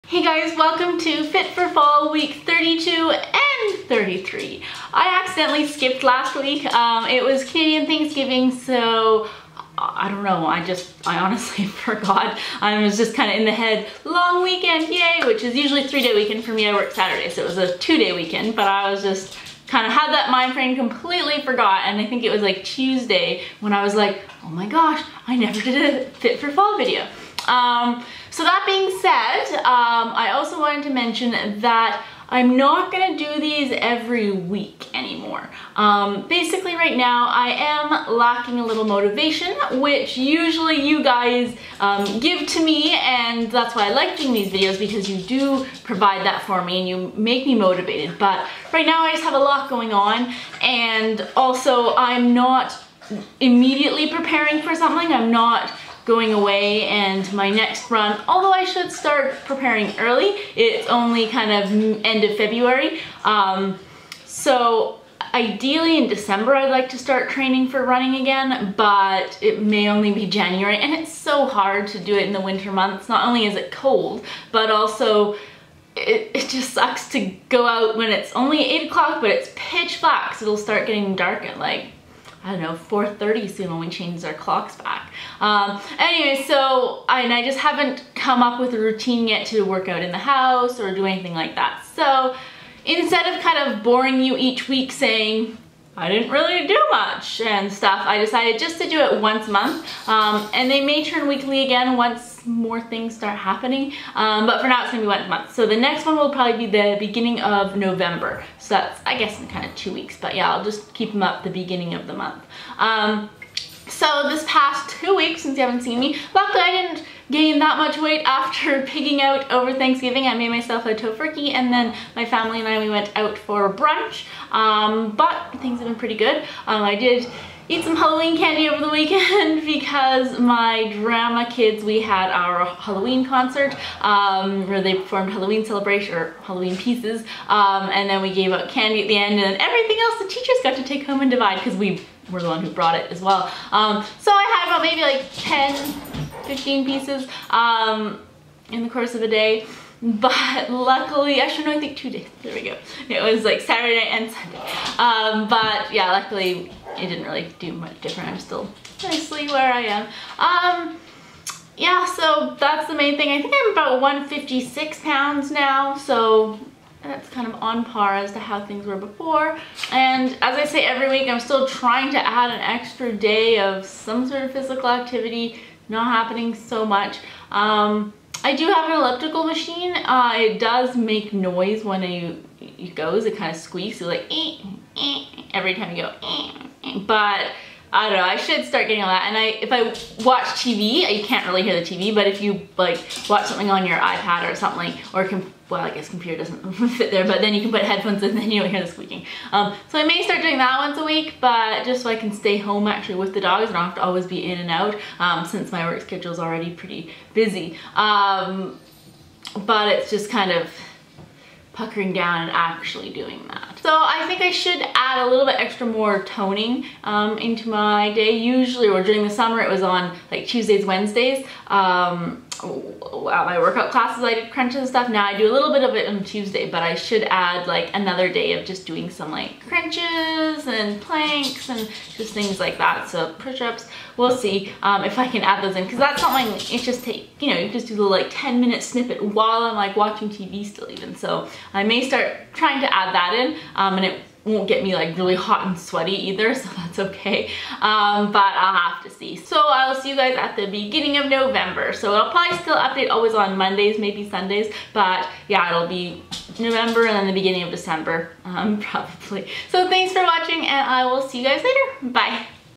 Fit For Fall Week 32/33 - Change In Schedule
Fit For Fall Week 32 and 33 (Oops, I missed last week!) - New fitness update schedule!

Send me a card or postcard:

Christy Len
Victoria, BC
V8T5G2
Canada